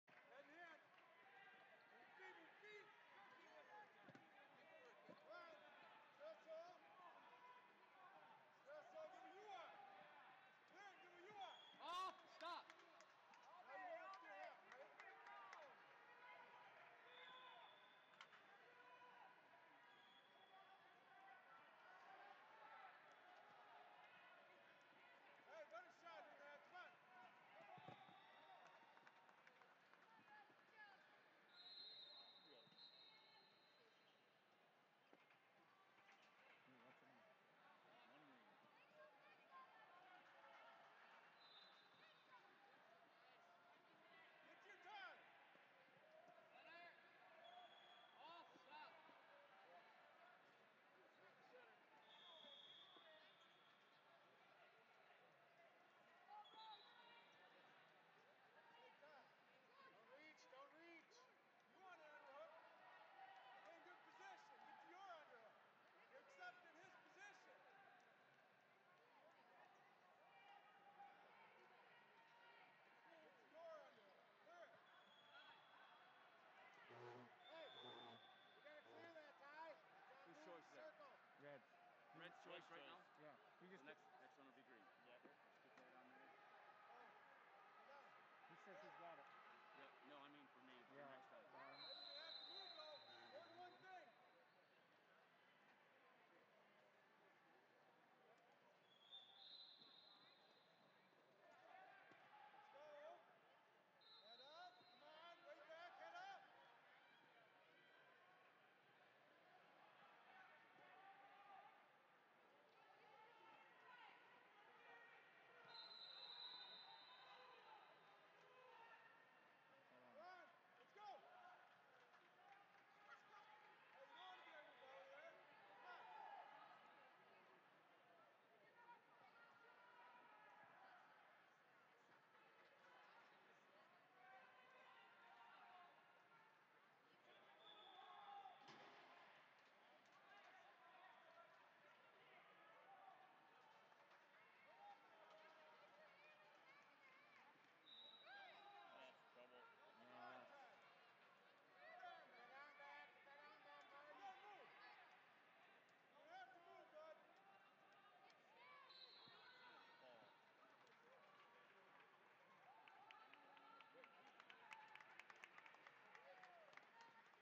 USAW TOA Club Duals HS Boys/[boutNo] 157 Tyson Martin LaCrosse Area Wrestlers Vs Bryce Burkett No
USAW TOA Club Duals HS Boys - USAW TOA Club Duals HS Boys/[boutNo] 157 Tyson Martin LaCrosse Area Wrestlers Vs Bryce Burkett No Nonsense Wrestling Red